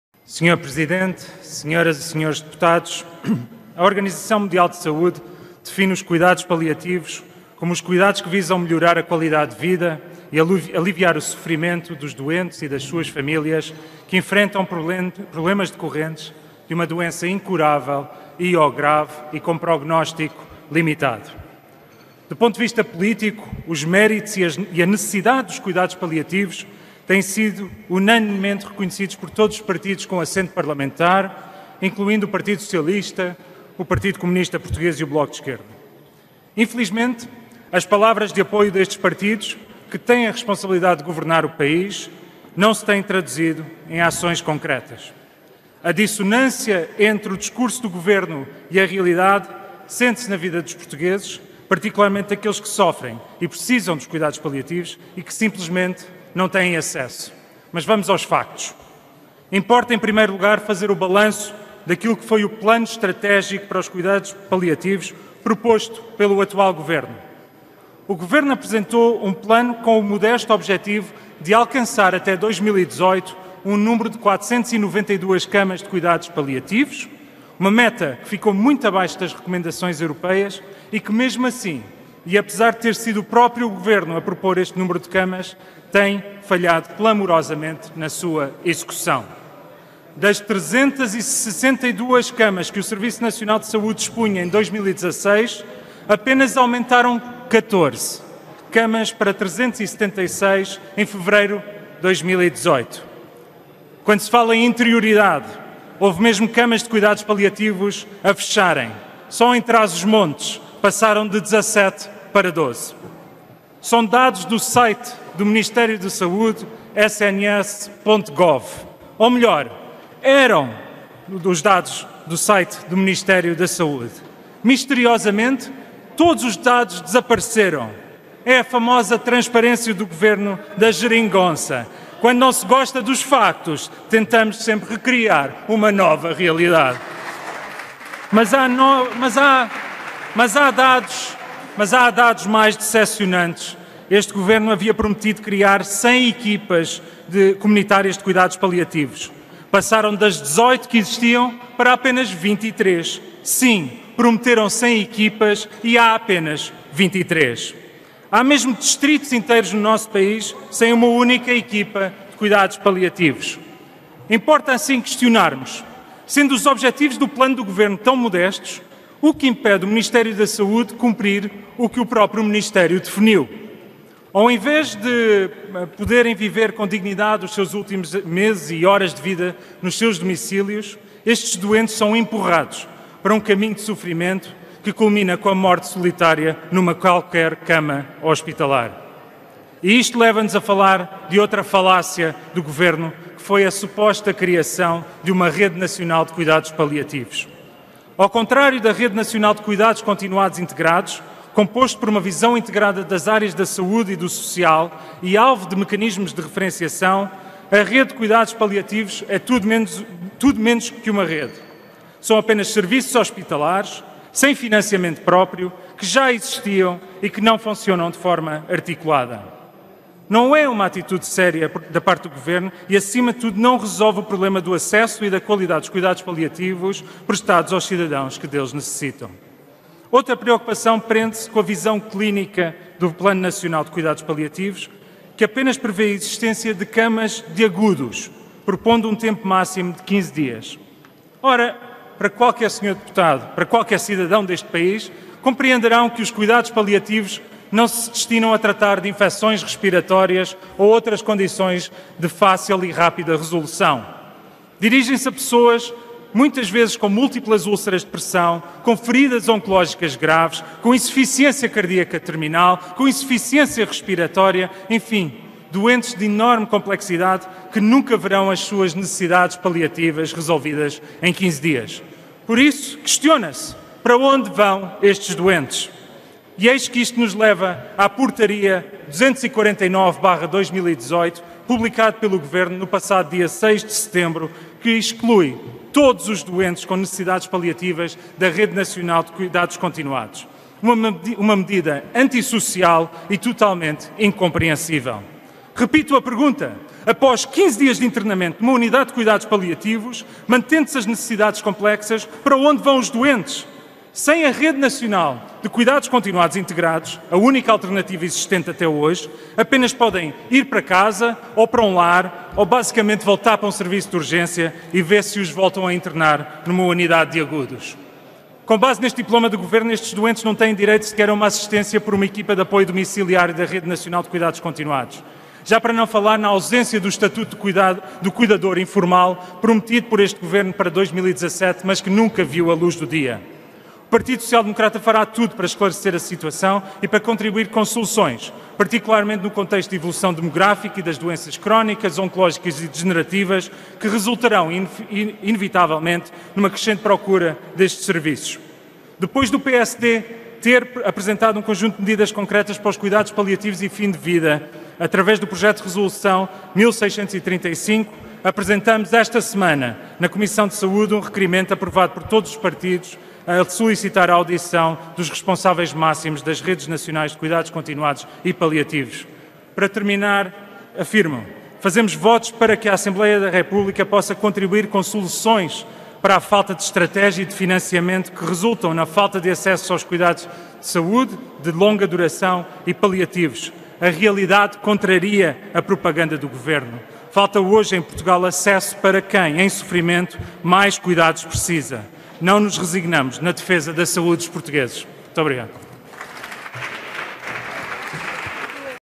Ricardo Baptista Leite: “a realidade contraria a propaganda do governo”
Numa declaração política em nome do PSD, Ricardo Baptista Leite considerou “antissocial e totalmente incompreensível” a decisão do governo de excluir todos os doentes com necessidades paliativas da Rede Nacional de Cuidados Continuados.